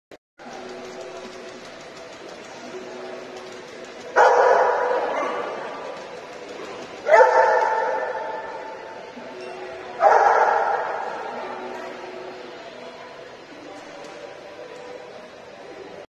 La Universidad de Málaga y Politécnica de Madrid crean 5 perros robóticos para localizar a víctimas
La Universidad de Málaga y la Politécnica de Madrid, además de empresas del sector, diseñan y crean cinco perros robóticos para localizar a víctimas junto con un perro real.